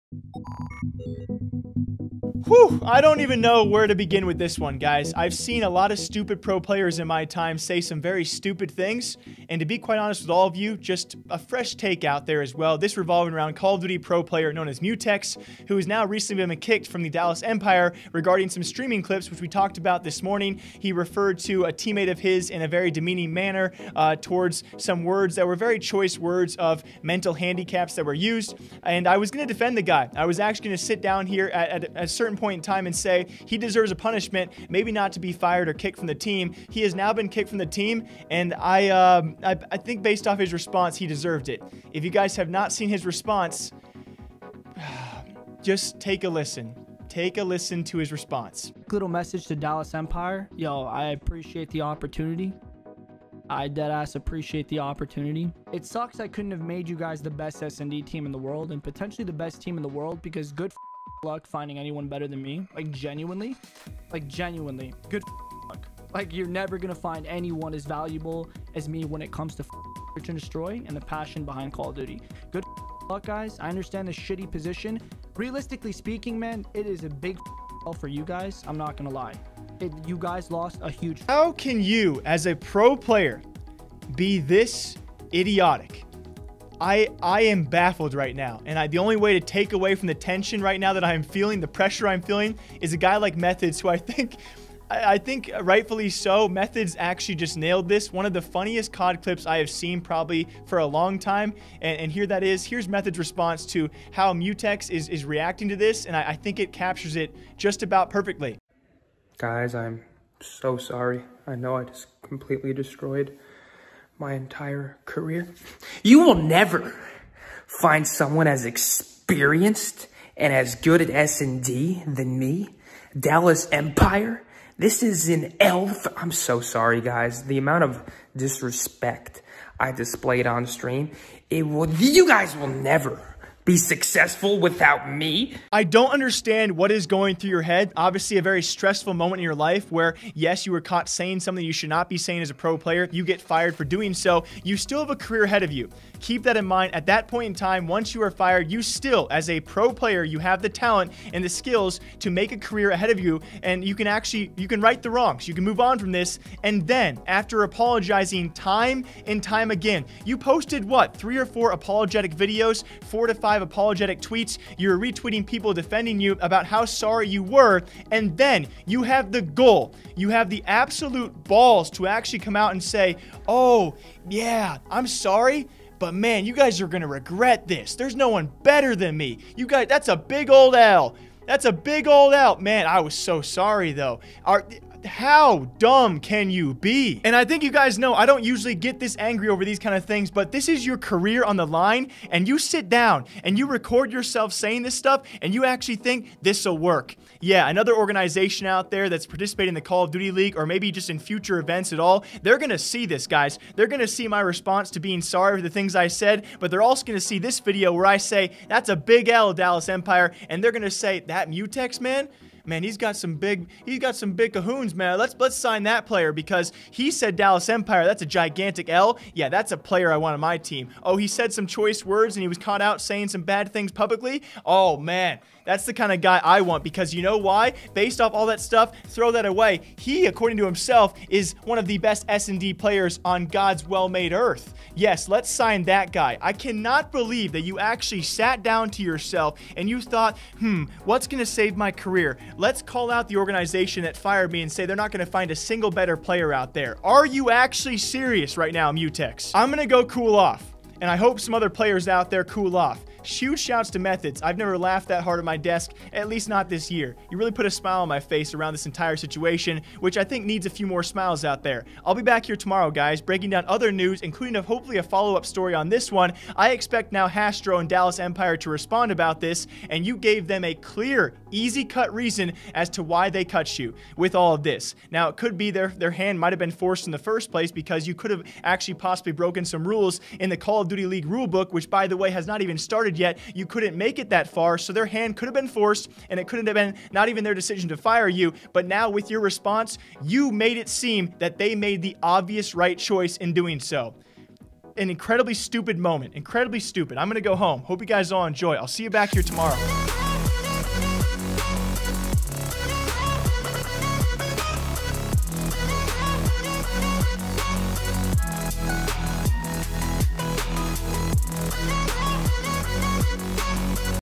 Mutex Just Ruined His Call of Duty Career
CoD Pro "Mutex" has responded to being kicked from the Dallas Empire with possibly the most interesting response I have ever heard. But also a response which could easily ruin his career.